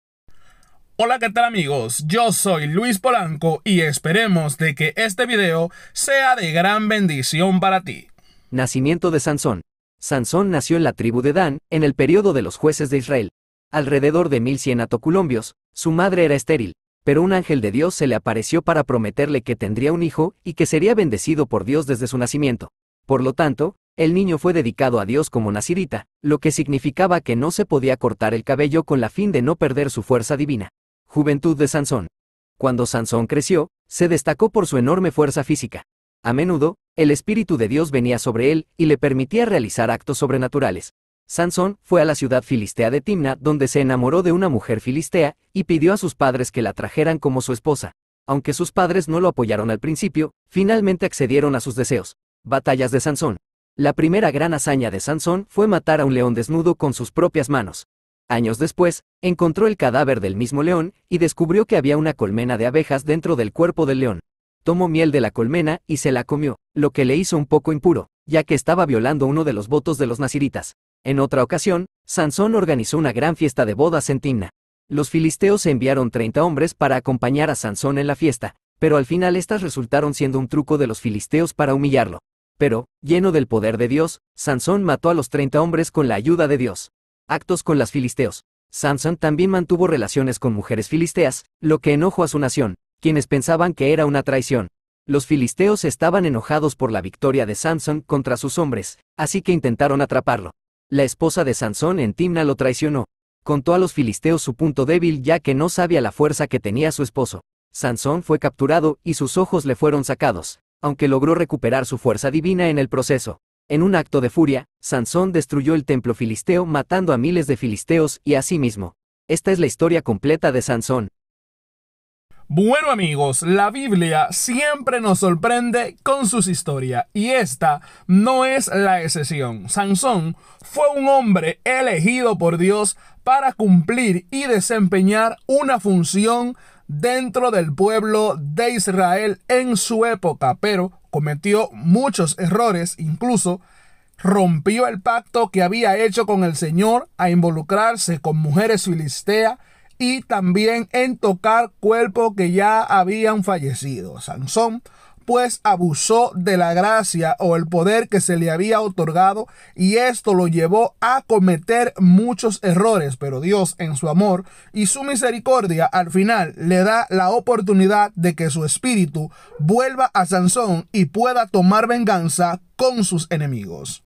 La Última Jugada de Samsón / Una maravillosa historia bíblica. BIOGRAFÍA DE SAMSÓN EN ESPAÑOL 2024
la historia de Samsón